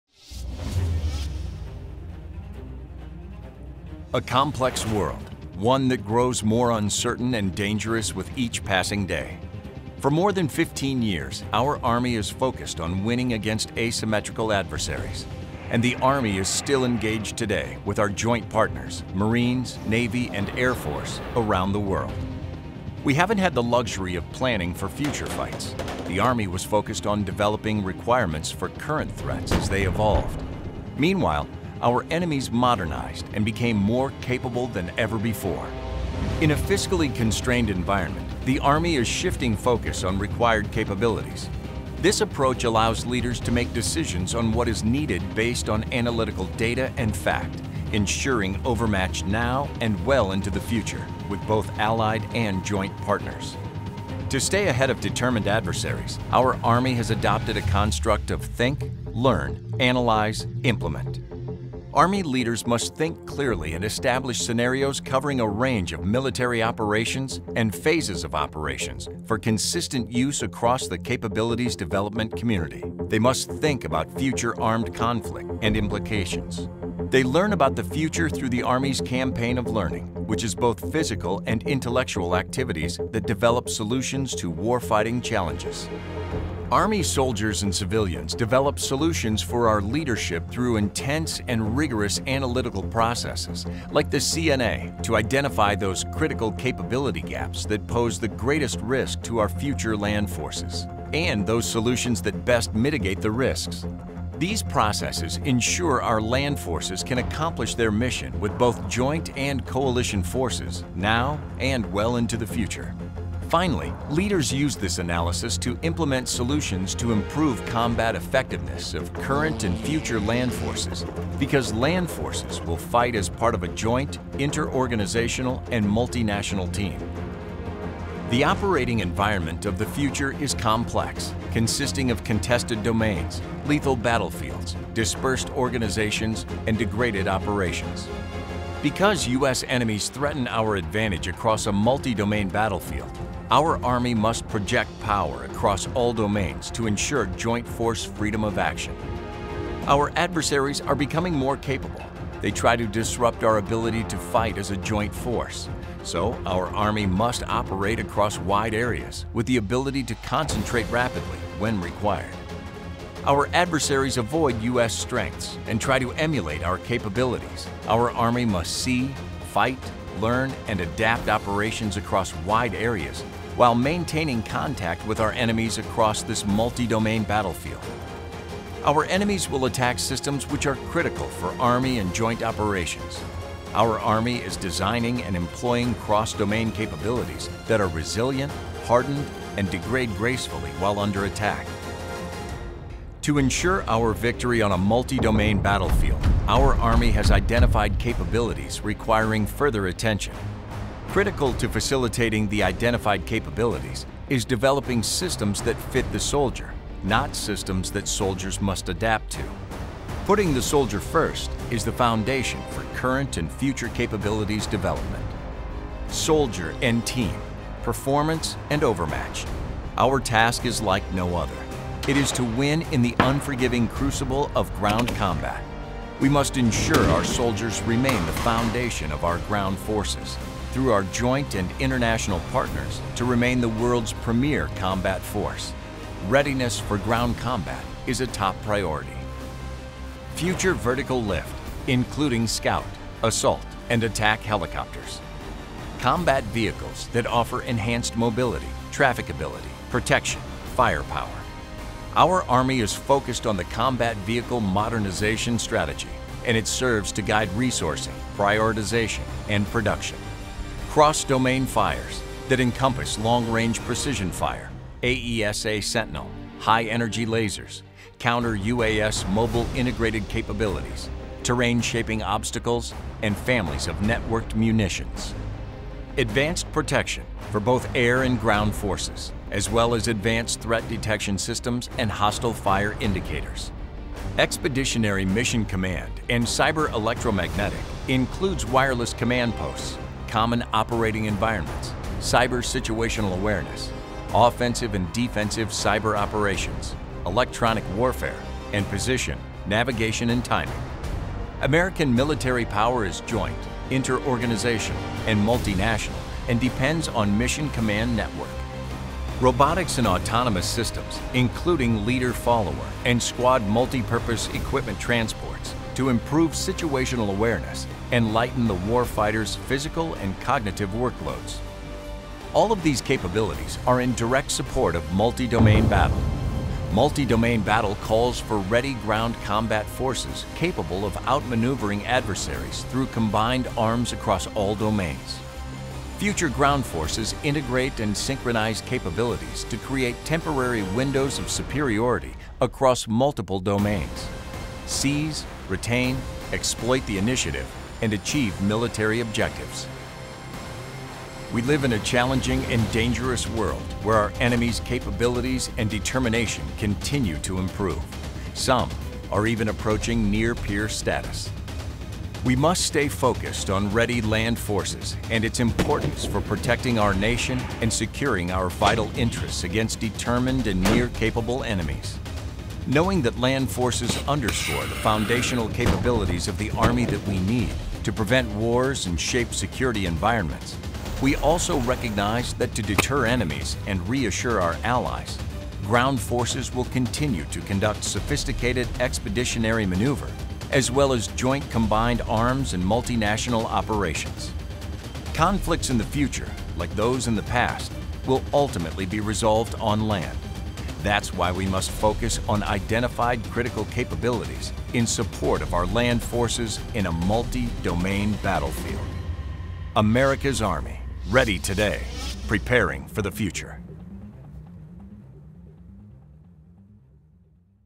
Multi-Domain Battle: Ensuring Joint Force Freedom of action
For more than 15 years, the Army has focused on winning against asymmetrical adversaries; however, the operating environment of the future is complex. Because U.S. enemies threaten the advantage across a multi-domain battlefield, the Army must project power across all domains to ensure joint force freedom of action. Learn how the Army will win in a complex world through the Multi-Domain Battle Concept, a concept that calls for ready ground combat forces capable of outmaneuvering adversaries through combined arms across all domains.